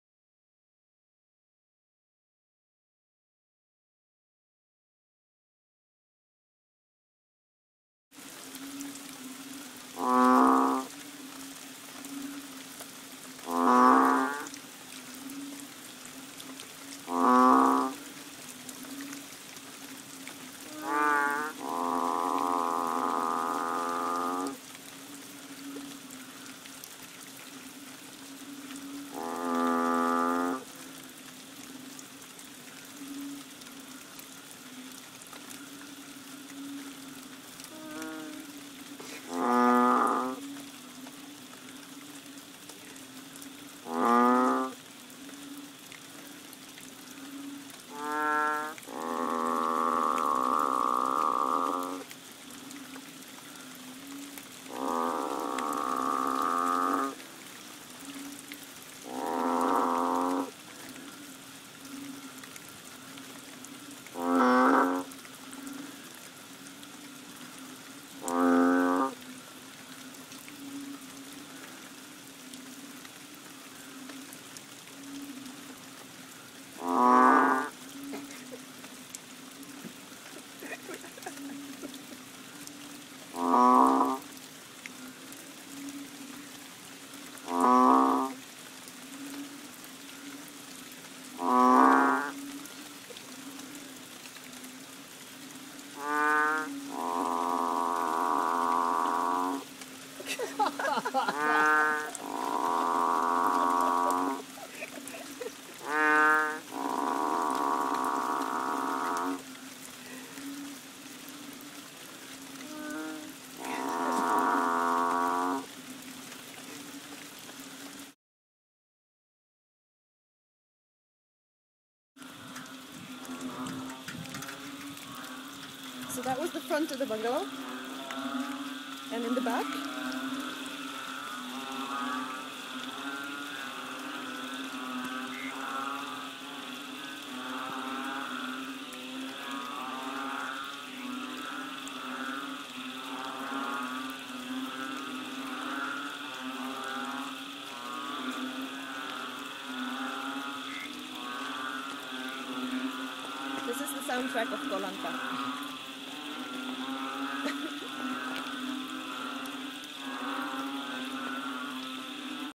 The Amazing Moo Frogs of Koh Lanta!
These frogs messed with my head during the rains in Thailand in March 2011. The two first ones were just outside the door, the chorus line was behind the bungalow. Camera shake is due to laughter...

Update: The frogs are probably of the banded bull frog species